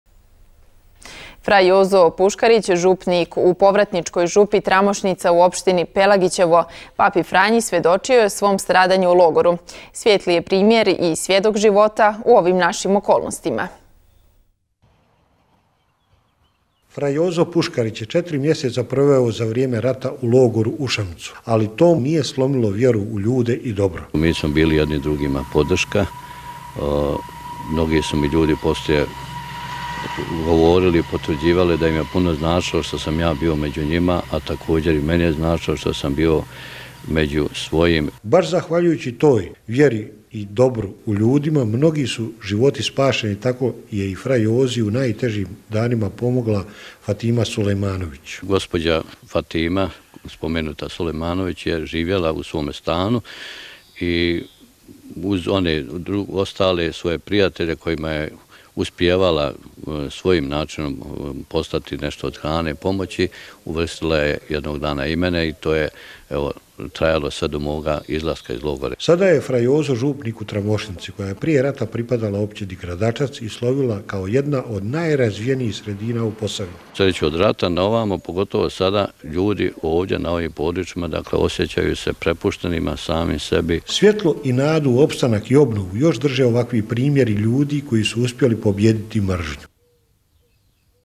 Svjedok
Fra Jozo Puškarić župnik u povratničkoj župi Tramošnica, u općini Pelagićevo, papi Franji svjedočio je o svom stradanju u logoru, a svijetli je primjer i svjedok života u ovim našim okolnostima.

Federalna.ba